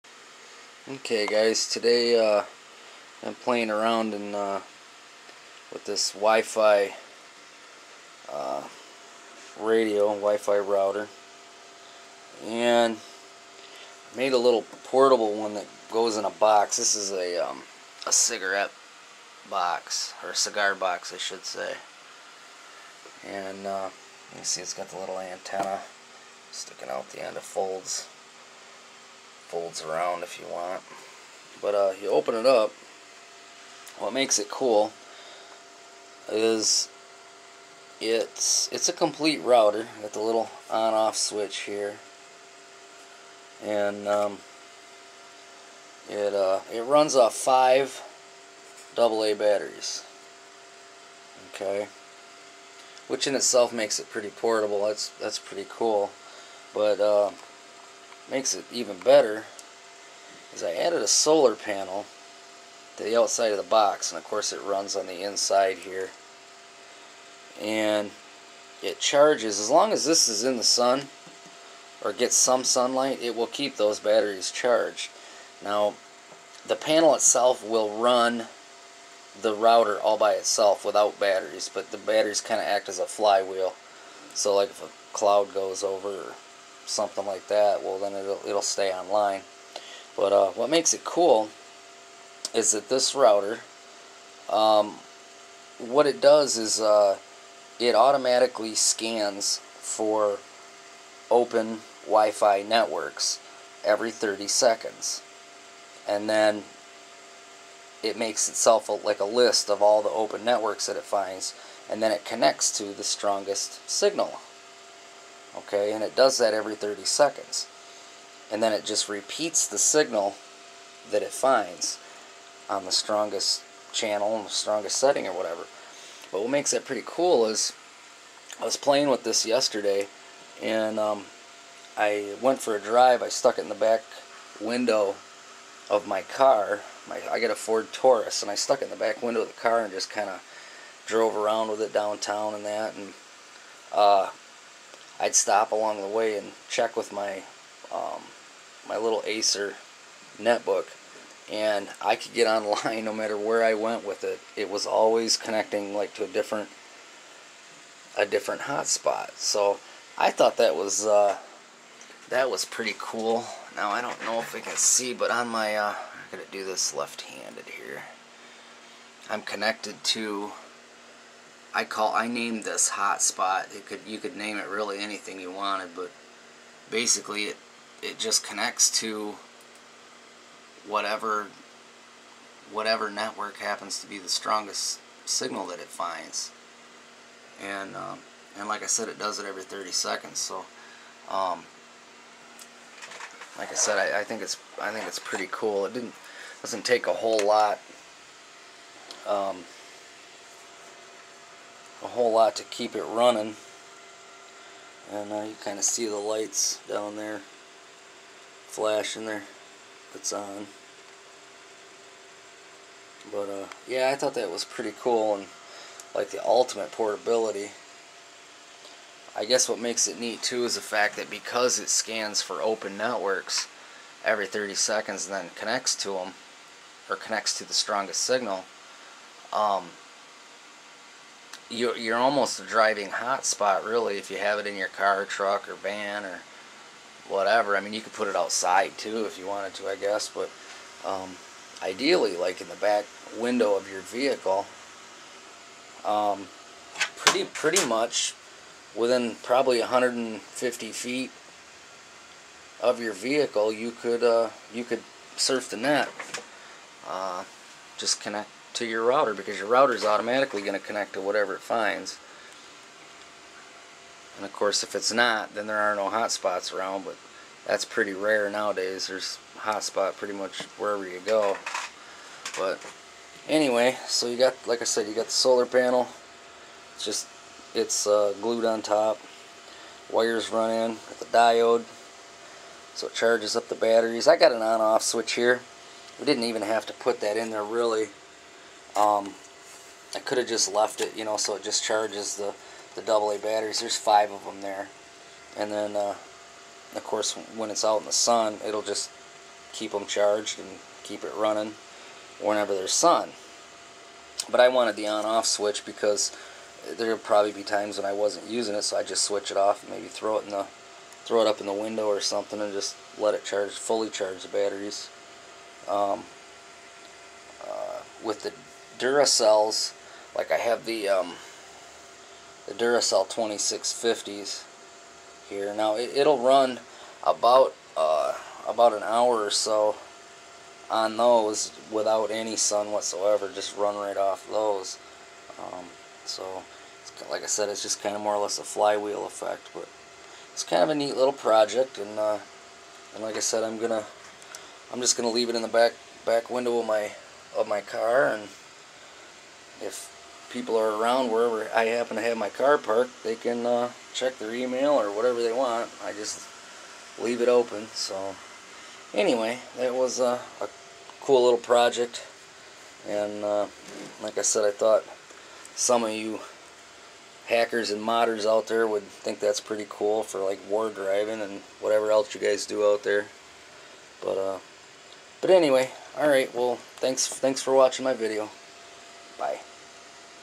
Solar Powered DIY Wi-Fi HotSpot
for more info. This is a portable wifi hotspot powered by a solar panel and 5 AA batteries. It automatically scans for open networks every 30 seconds and then connects to the strongest signal it finds. It then repeats that signal so you can surf the net locaally for up to about 150 feet or more. I put this in the back window of my car and leave it. No matter where I drive me and others are able to connect to it and check email, surf, etc. If you would like me to custom build one of these for you please Visit the to contact me. Thanks